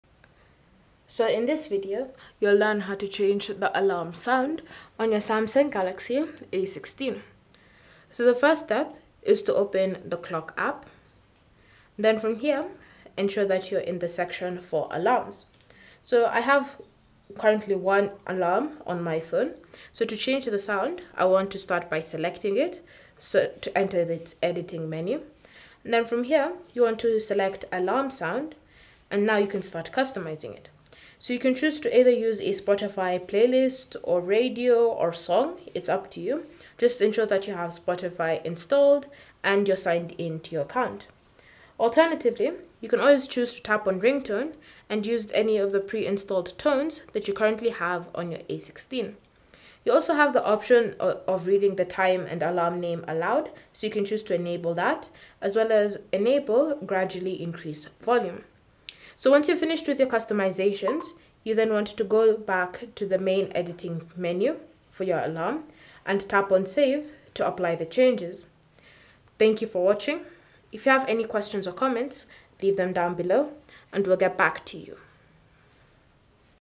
How To Change Alarm Sound On Samsung Galaxy A16
Learn how to change alarm sound on Samsung Galaxy A16 to personalize your wake-up experience.

Whether you prefer a soothing melody or a more energetic tone, this guide will walk you through the simple steps to update your alarm sound. By adjusting the sound settings, you can make your mornings more pleasant and start your day with a sound that suits your preference.

Open the Clock app and go to the Alarms section.

Select the alarm you want to edit to enter the alarm settings.

Tap on "Alarm Sound" to start customizing your alarm tone.

You can choose a Spotify playlist, radio, or song if you have Spotify installed and signed in.

Alternatively, you can select "Ringtone" to use any of the preinstalled tones.

You also have the option to enable "Read time and alarm name aloud" and "Gradually increase volume."

After making your customizations, tap "Save" to apply the changes.

Cell Phone Tripod Adapter